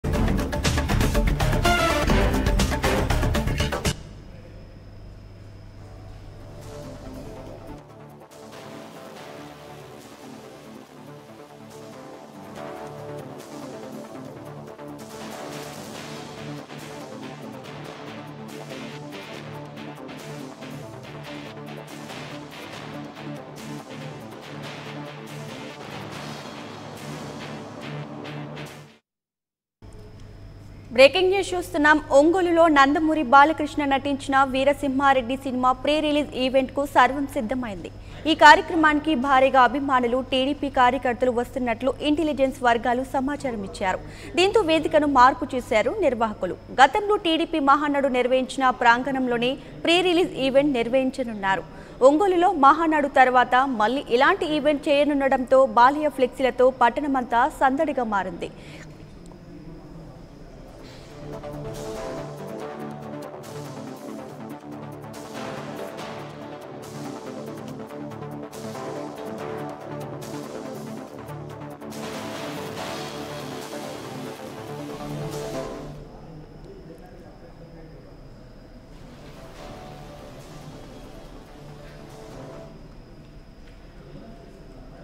Breaking News : బాలయ్య ఫ్లెక్సీలతో కళకళలాడుతున్న ఒంగోలు | 99TV
99TV Telugu LIVE | AP News | Telangana News | 99TV Telugu Live TV Channel